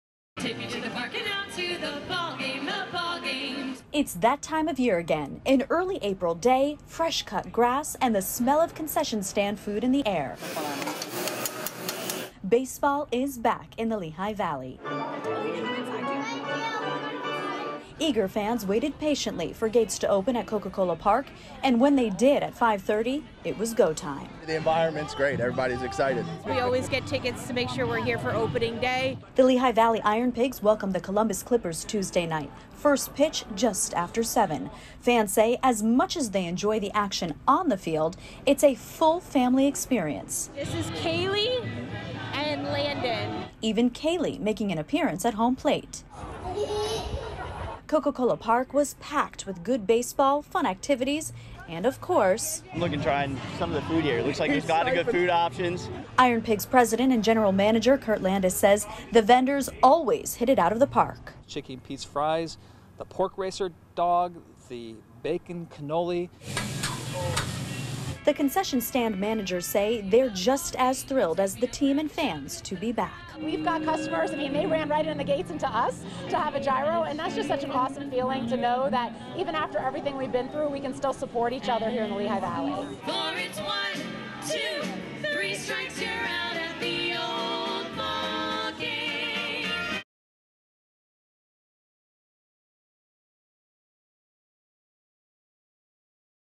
Iron Pigs Opening Day
2022 Opening day of Baseball Season at Coca-Cola Park in Allentown PA.